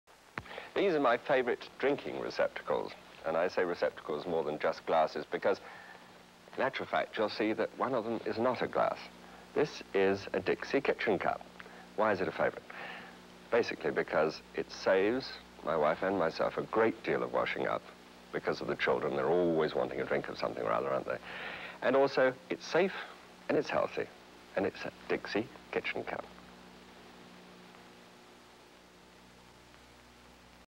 Dixie Cups 1970 ad "Galloping Gourmet: Kitchen Cups 2" :30, from Georgia-Pacific
In this ad from 1970, celebrity chef Graham Kerr (a.k.a. The Galloping Gourmet) takes us on a tour of his favorite drinking receptacles - not glasses - and explains why labor-saving, sanitary Dixie Cups are one of his top choices.

This is the second of 5 commercials that The Galloping Gourmet did for Dixie Cups in 1970.  He hosted an Emmy-nominated, humor-based and butter-laden cooking show on television from 1969 to 1971, and many specials and programs thereafter.

Mr. Kerr's personal website and current ideas about health and cooking can be found